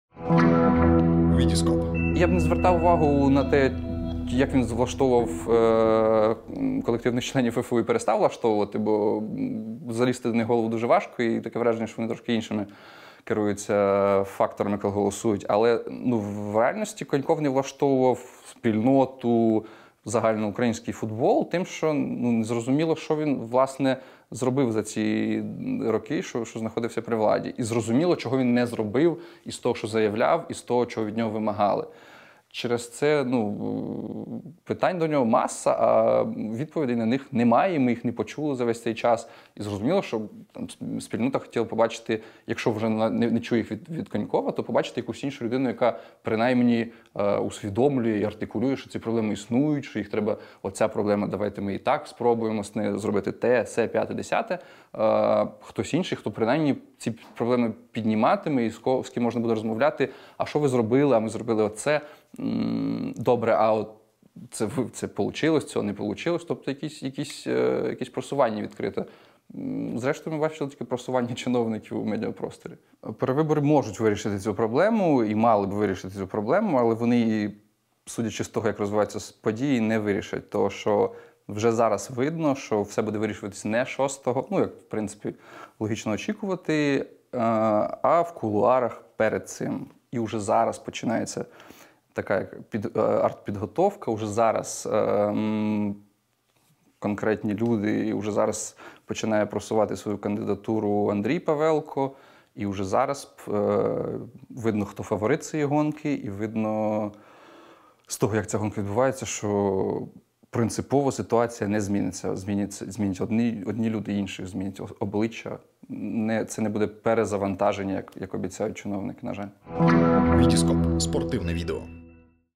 Олександр Ткач про можливі перевибори президента ФФУ
Головний редактор Трибуни Олександр Ткач ділиться своїми думками з приводу анонсованого перезавантаження Федерації футболу України та виборів президента ФФУ.

Я би не звертав увагу на те як Коньков влаштовував увагу колективних членів ФФУ, а потім перестав влаштовувати. Залізти до них у голову дуже важко і таке враження, що вони трошки іншими керуються факторами коли голосують, але в реальності Коньков не влаштовував спільноту, загальноукраїнський футбол, тому що не зрозуміло що він зробив за ці роки, що він був при владі, зрозуміло чого він не зробив із того, що заявляв і чого від нього вимагали.

Питань до нього маса, але відповіді на них немає, ми їх не почули за весь цей час. Зрозуміло, спільнота хотіла побачити, якщо вже не почула їх від Конькова, побачити людину, яка принаймні їх артикулює, їх усвідомлює, яка принаймні говорить, ось ця проблема, давайте з нею ось це зробимо, те-се п’яте-десяте. Хтось інший, хто ці проблеми підніматиме, з ким можна розмовляти: а що ви зробили? Ми зробили ось це. Добре, це получилося, цього не получилося, тобто якесь просування відкрите. Зрештою ми бачили тільки просування чиновників у медіа просторі.

Перевибори можуть вирішити цю проблему і вони мали би вирішити цю проблему, але судячи з того як розвиваються ці події, вони не вирішать проблему. Тому що вже зараз видно, що все буде вирішуватися не шостого березня, як це логічно, а в кулуарах перед цим. Вже зараз розпочинається артпідготовка така. Уже зараз конкретні люди, і уже зараз починає просувати кандидатуру свою Андрій Павелко, і вже зараз видно, хто фаворит цієї гонки. Видно, що принципово ситуація не зміниться. Зміняться обличчя, одні люди змінять інших, це не буде перезавантаження, як обіцяють чиновники, нажаль.